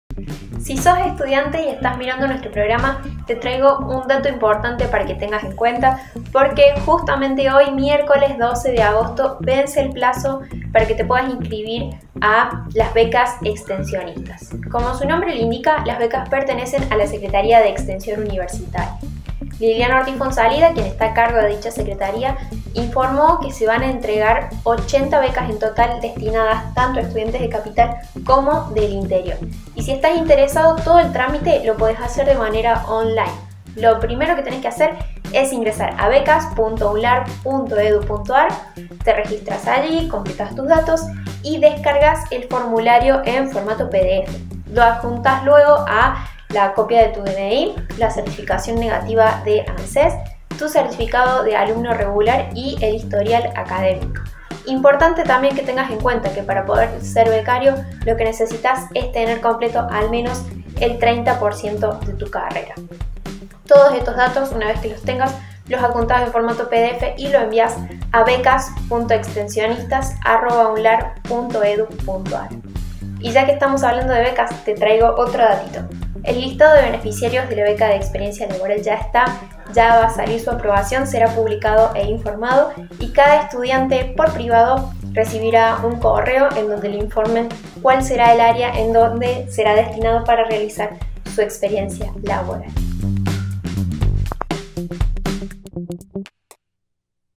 Cómo inscribirse en las becas extensionistas
Natalie Leautier informa sobre los requisitos y la modalidad de la convocatoria abierta para becas de extensión en la UNLaR.